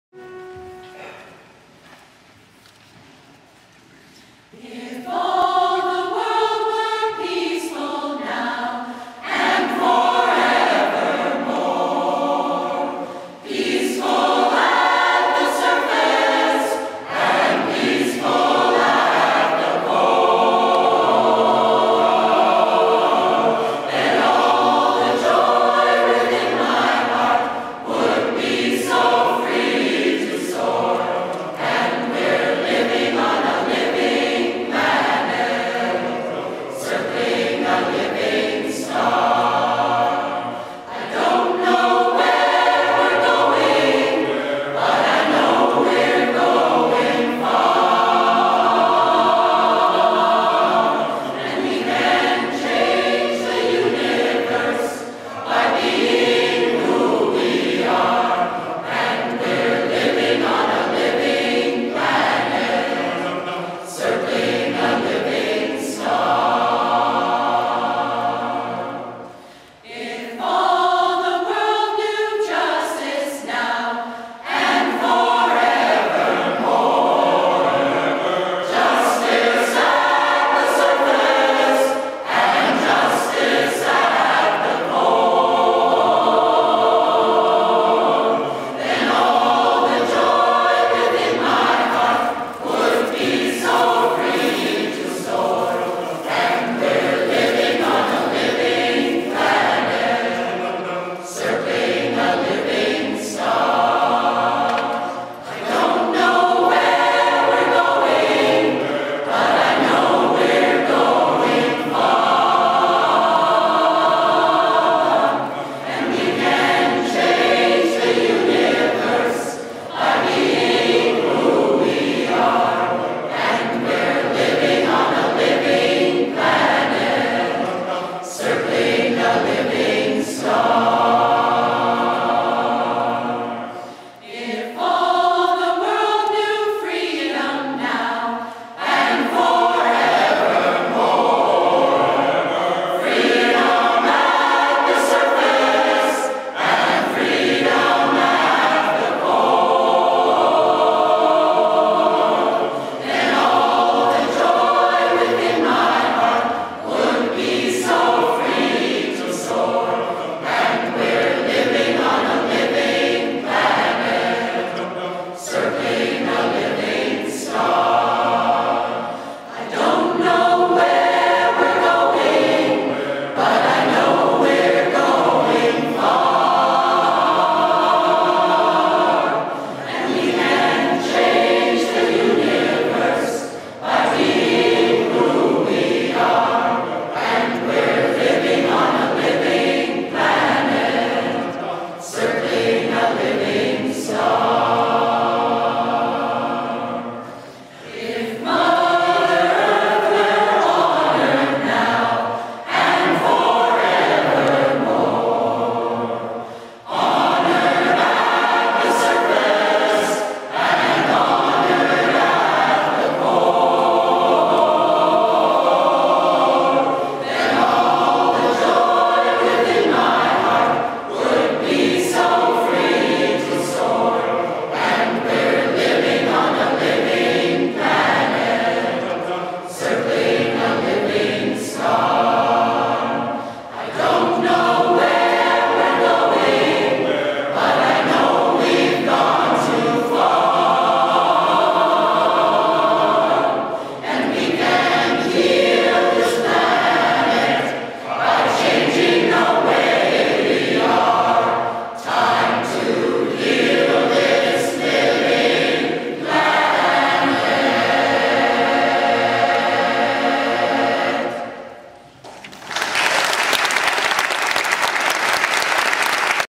Living Planet 2010 Common Thread Community Chorus of Toronto
Common Thread Community Chorus of Toronto. Alan Gasser, Guest Conductor. Recorded on Saturday, May 29, 2010 at Eastminster United Church, Toronto, Ontario, Canada.